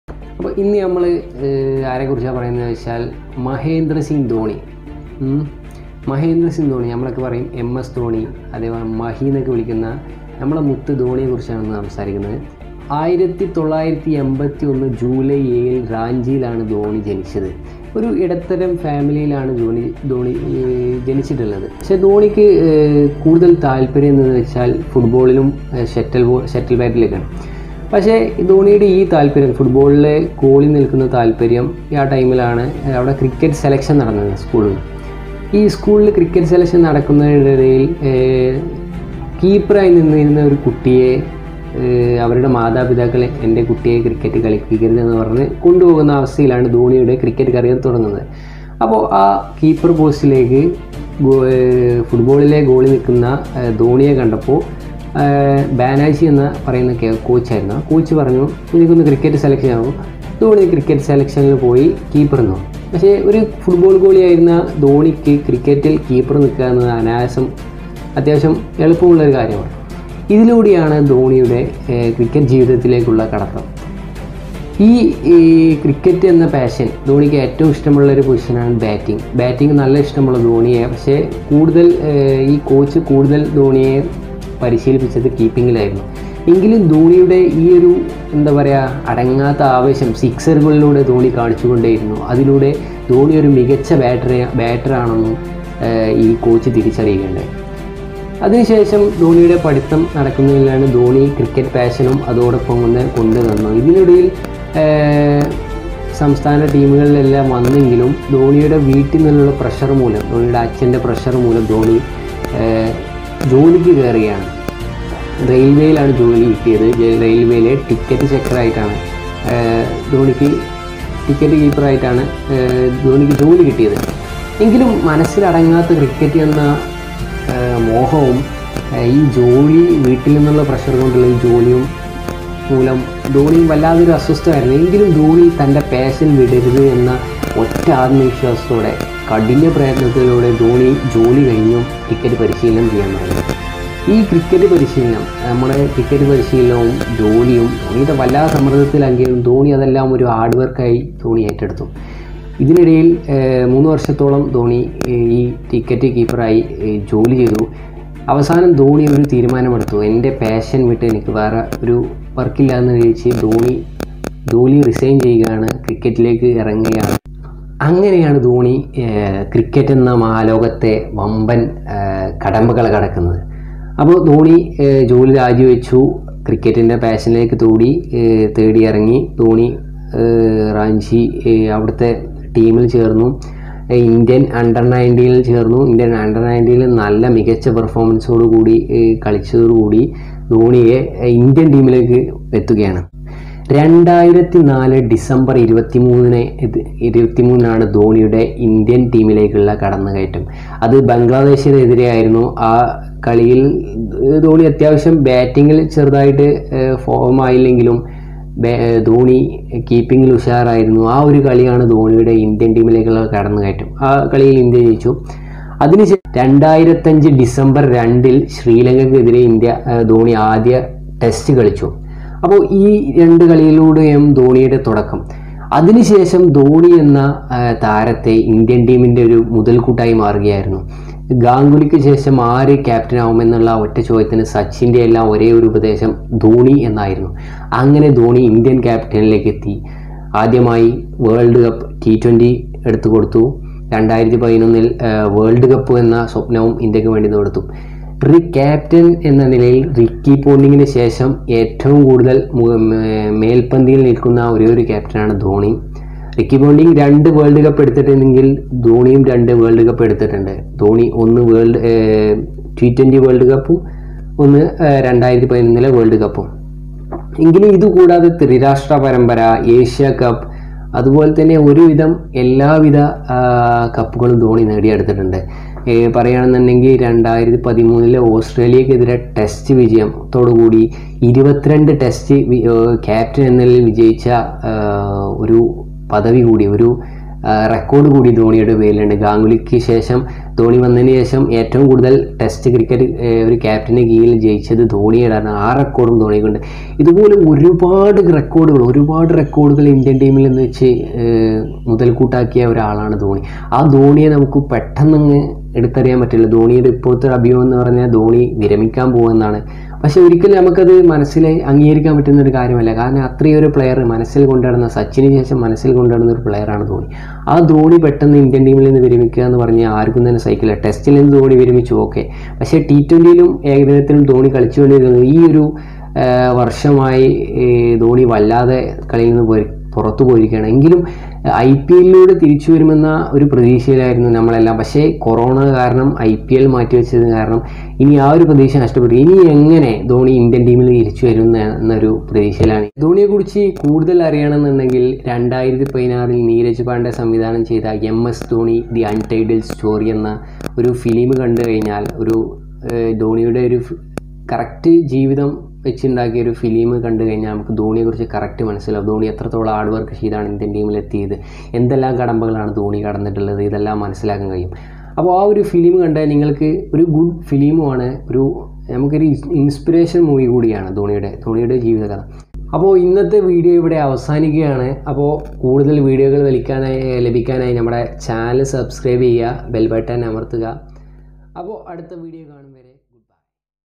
MS DHONI LIFE STORY | ധോണി ഇന്റർനാഷണൽ ക്രിക്കറ്റിൽ നിന്ന് വിരമിച്ചു 😔😔 | ധോനിയുടെ ജീവ ചരിത്രം
MS DHONI RETIREMENT
MS DHONI
DHONI LIFE STORY
INDIAN CRICKET
INDIAN CRICKETER